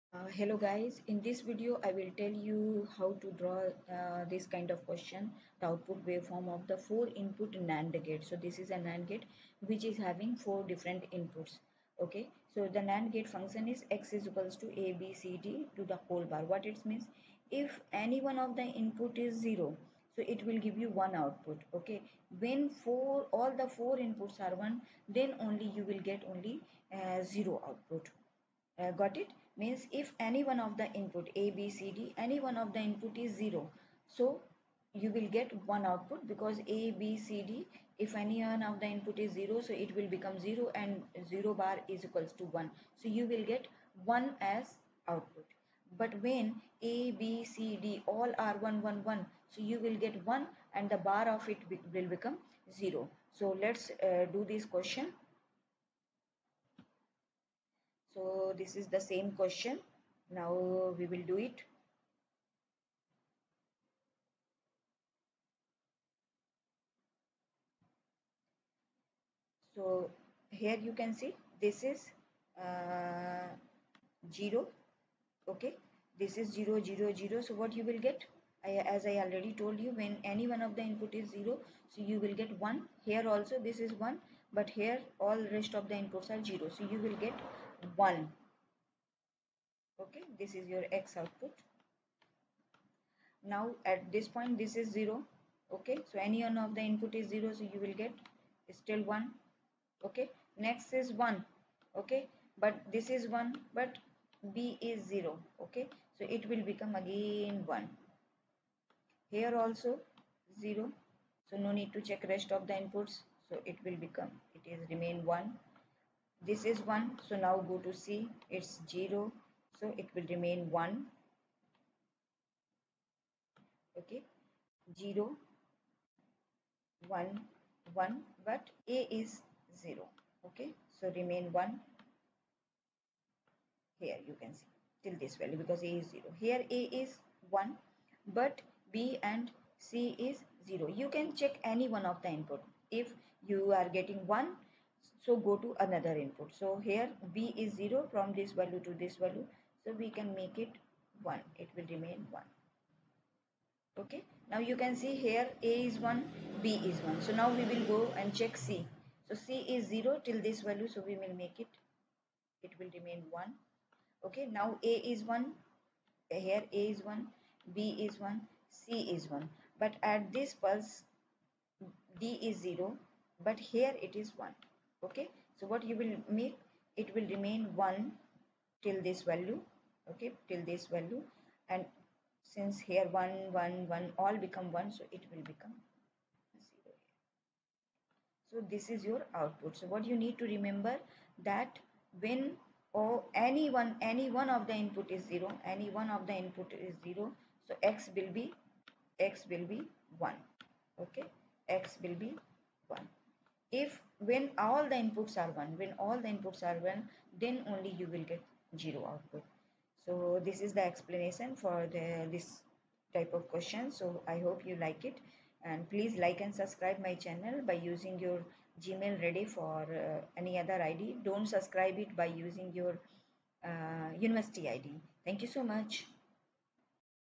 Output Waveform of NAND Gate/ Basic logic gate output waveform/Digital electronics
Draw the output waveform of four Input NAND Gate. The timing diagram of all four inputs A, B, C, and D is given, In this video, we will draw the output timing diagram of the NAND gate.